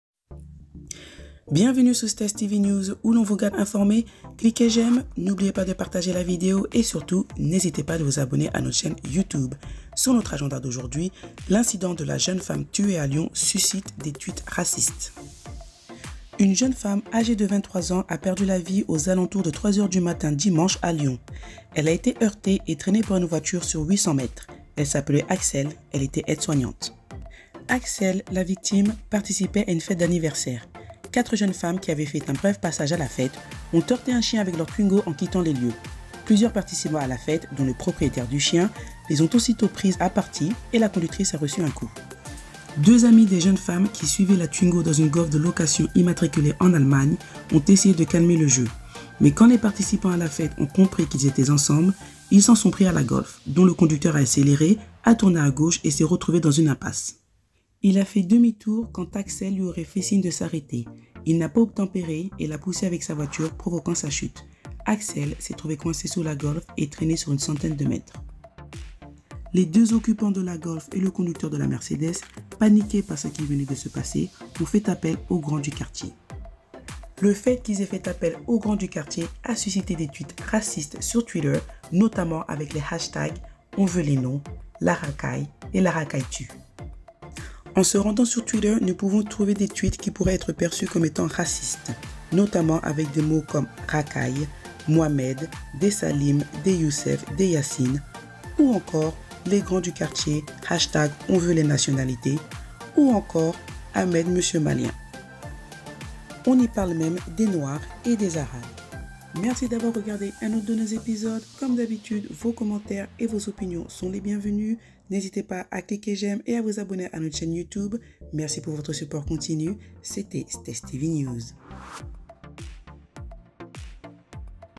[STESS TV News] Des Tweets Racistes Sont Publiés Suite à La Mort d'Axelle Dorier
[STESS TV News] Jeune Femme Tuée à Lyon, Des Tweets Racistes Sont Publiés